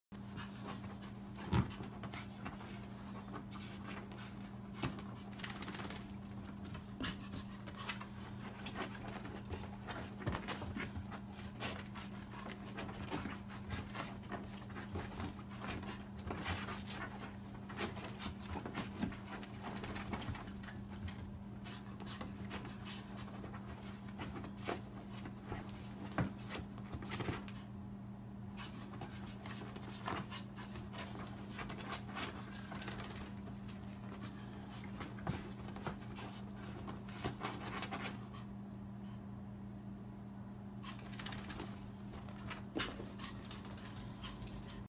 Zoe took a bath
The first time i gave my moms dog a bath gave this result